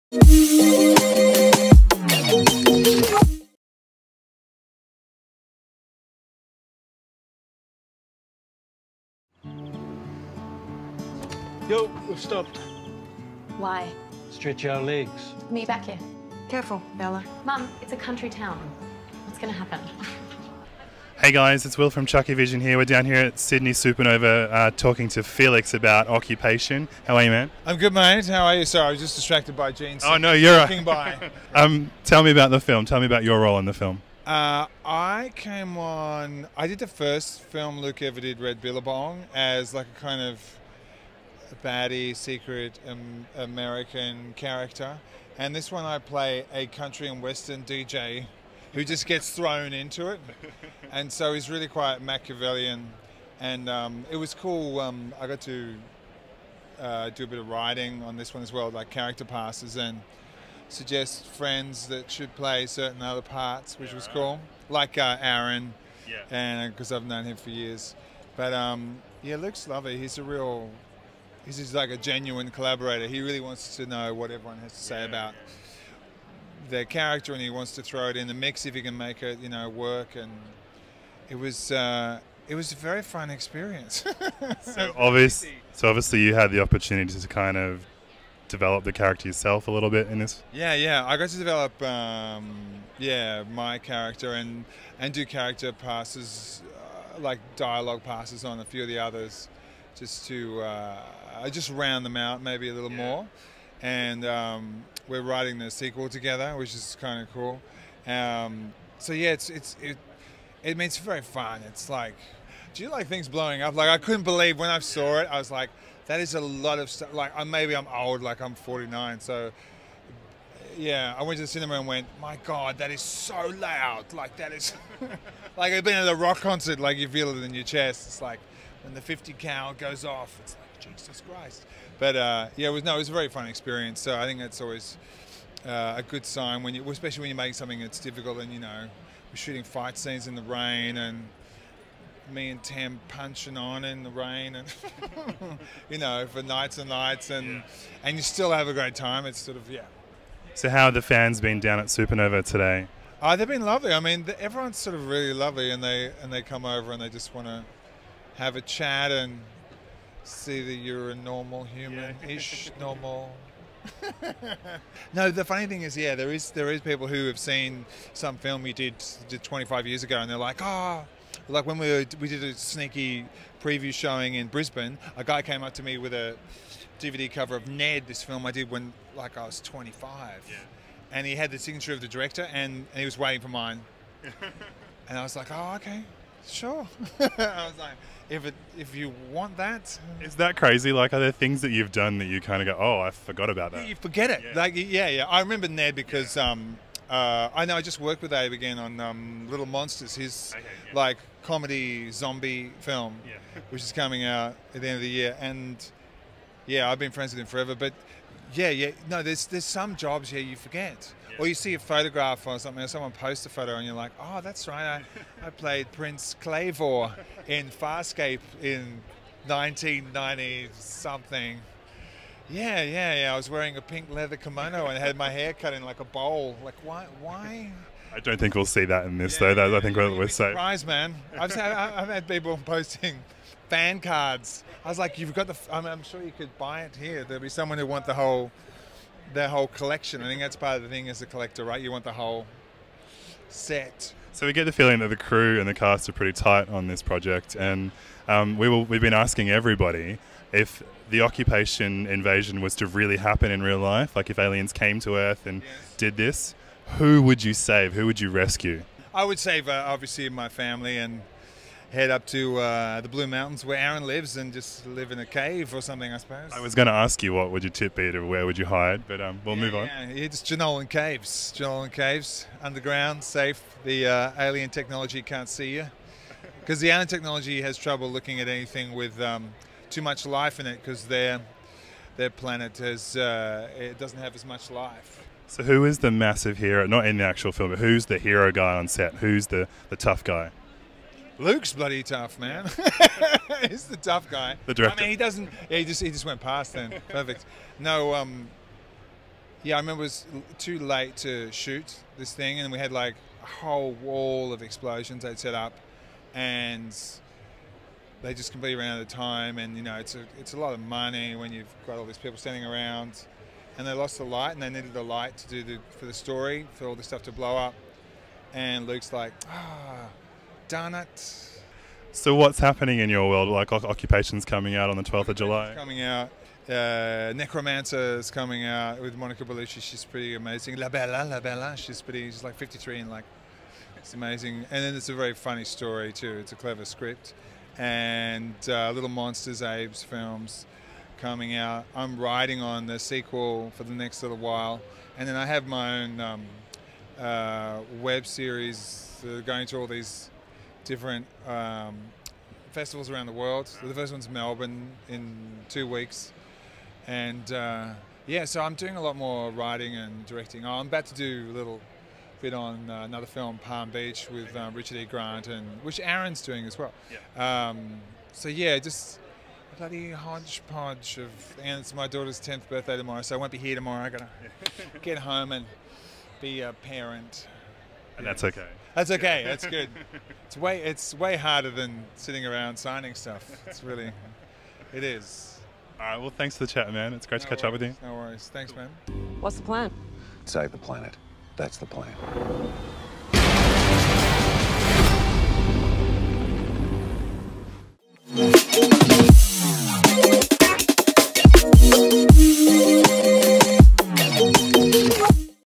Occupation the moive: Exclusive chat with Felix Williams at Sydney Supanova 2018
We chat to Felix Williams about his role in new Australian Sci-Fi film 'Occupation' which hits cinemas in Australia on July 12th!